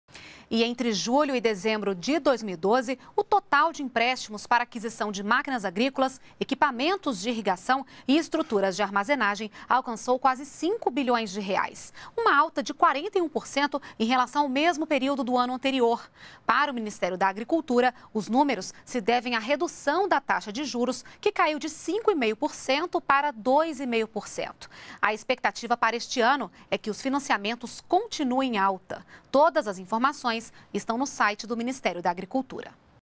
Aumenta número de empréstimos para o setor agrícola
NBR NOTÍCIAS - 25.01.13: Entre julho e dezembro de 2012, o total de empréstimos para aquisição de máquinas agrícolas, equipamentos de irrigação e estruturas de armazenagem alcançou quase R$ 5 bilhões. Uma alta de 41% em relação ao mesmo período do ano anterior. Para o Ministério da Agricultura, os números se devem
à redução da taxa de juros que cai de 5,5% para 2,5% ao ano. A expectativa para
2013 é que os financiamentos continuem em alta. Todas as informações estão no site do Ministério da Agricultura.